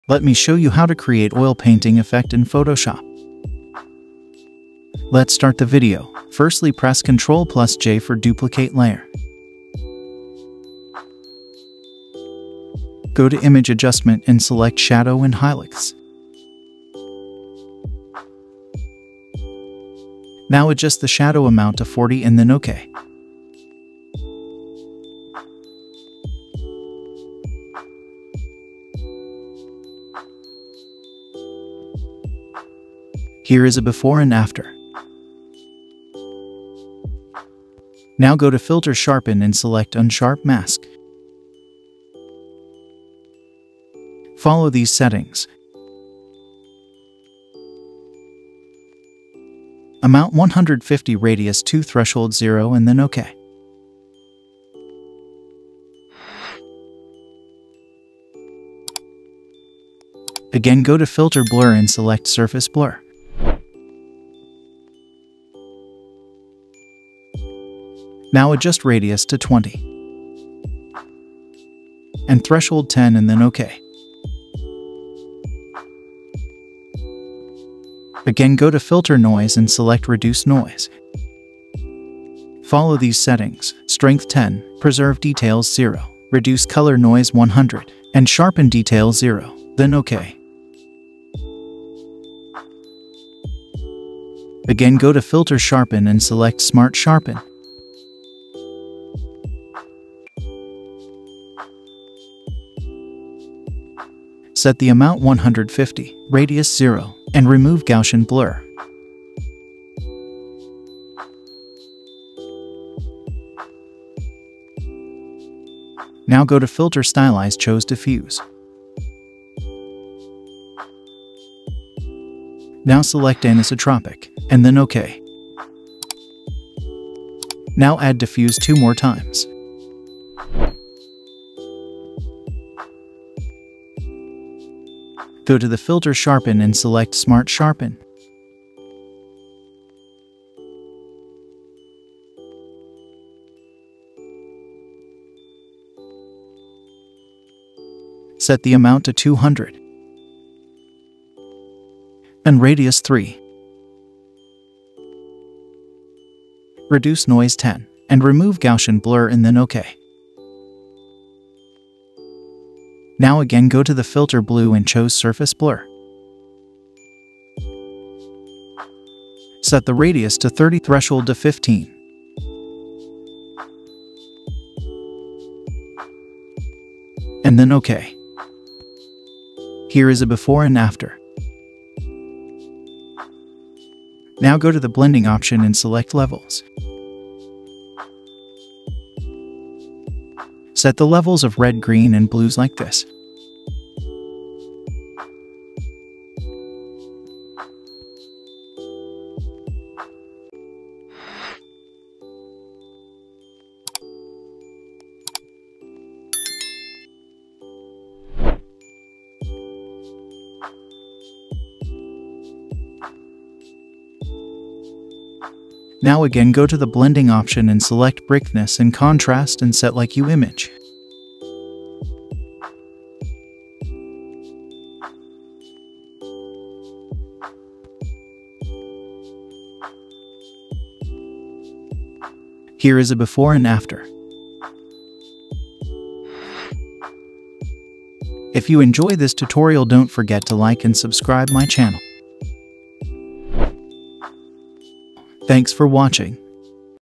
Oil Paint Effect in Photoshop Tutorial
In this tutorial, we're going to learn about Oil Paint Effect in Photoshop | Photoshop Tutorial. This tutorial is for beginners.
so guys see the Video. I hope this video will help beginners.